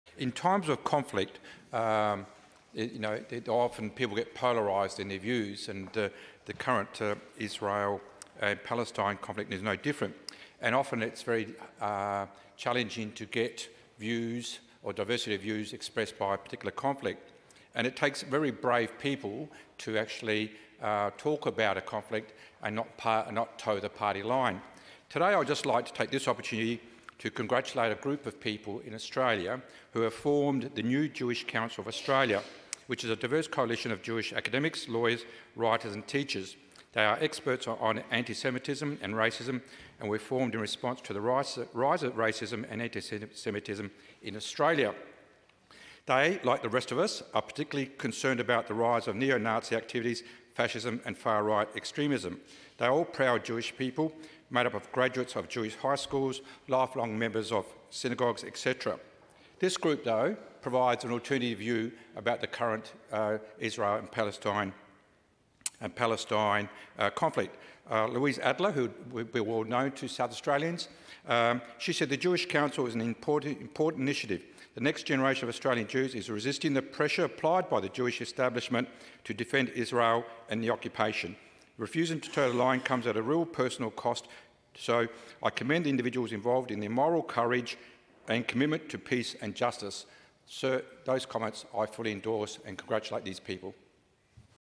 Jewish Council of Australia - 9 APR 2024
In times of conflict often people get polarised in their views, and the current Israel-Palestine conflict is no different. Often it is very challenging to have a diversity of views expressed by a particular conflict. It takes very brave people to actually talk about a conflict and not toe the party line.